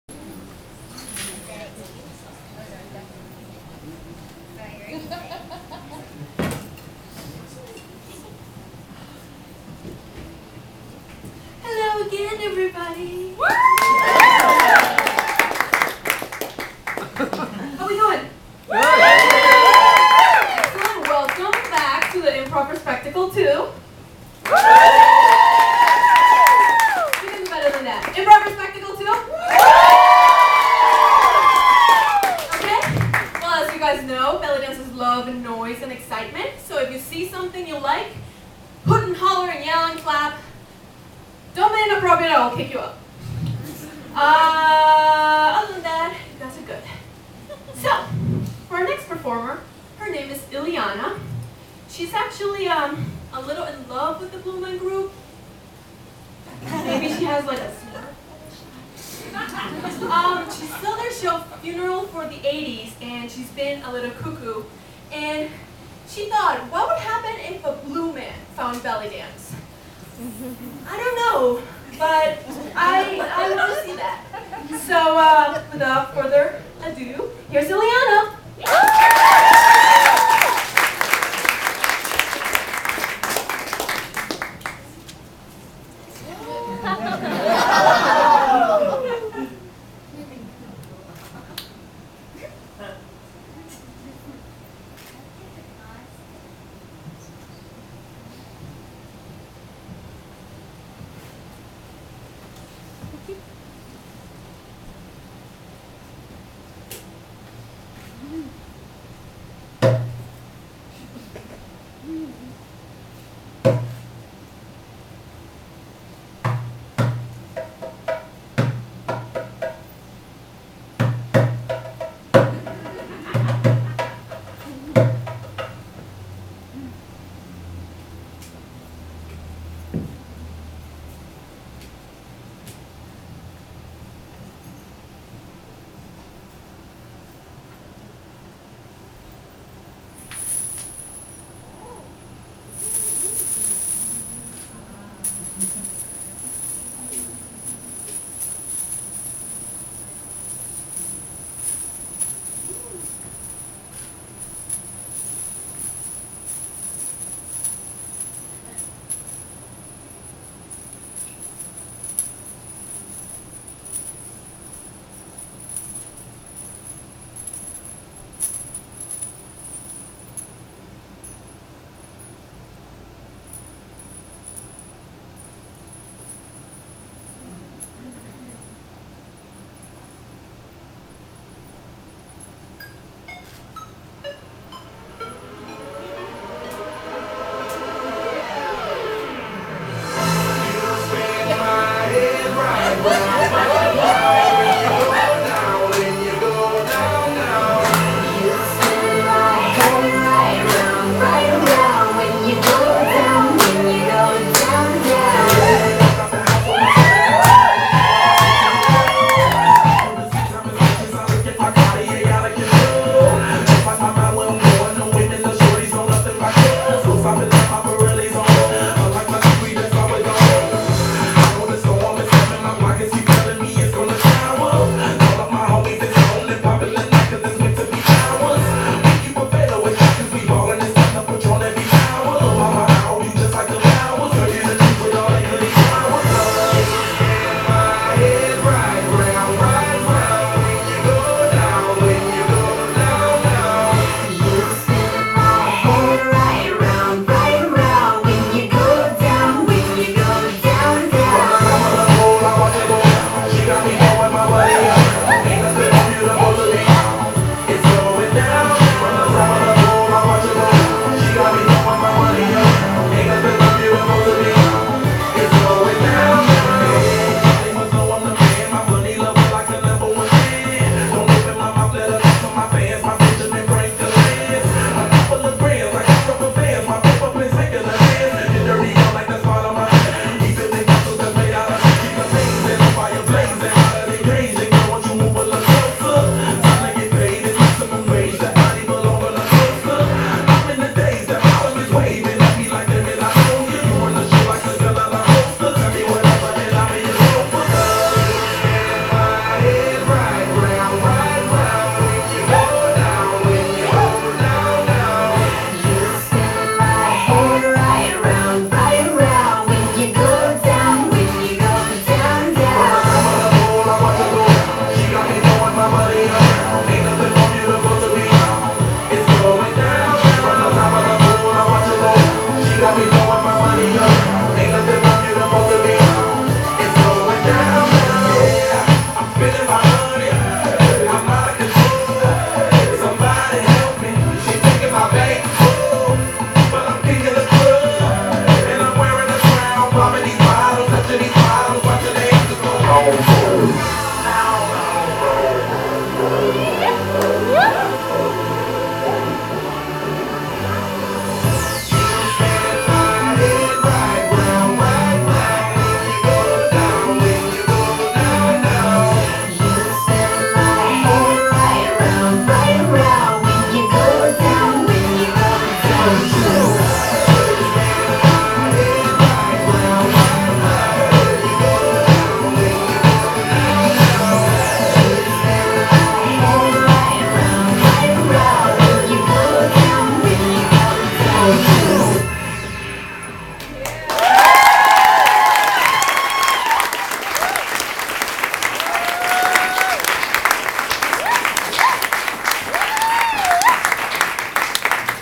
Blueman Group bellydancing
Bellydancer, Illianna, performing at the Improper Spectacle 2 - a belly dance tribute to the Blueman Group.